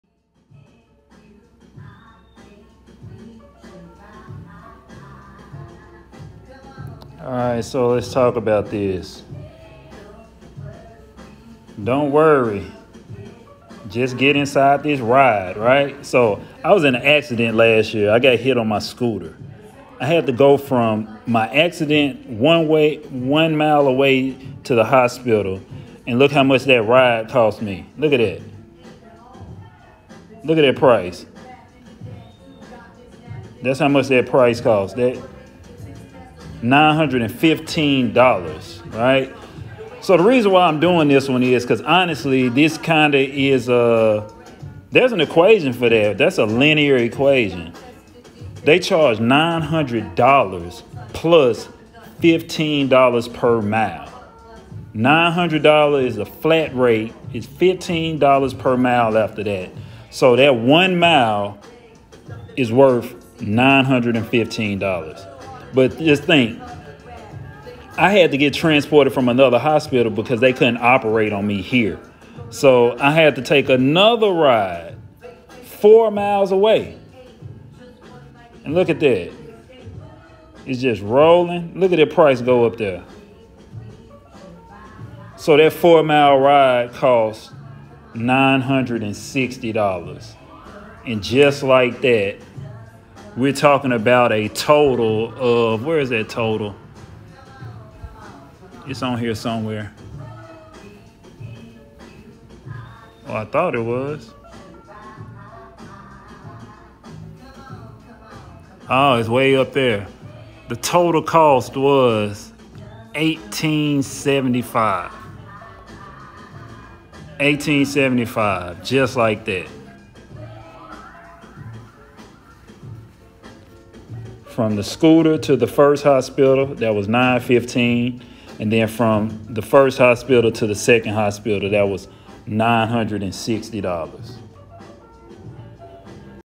Real life situation that happened to me 9/19/2019...🚑🏥
I was in an accident last year that cause me to take off from work from 9/19/2019 to 1/6/2020. I think silly enough to take such a bad situation and use it as a teachable moment in linear equations.

Make sure you have insurance please...I’m glad I don’t have to pay 100% of the costs.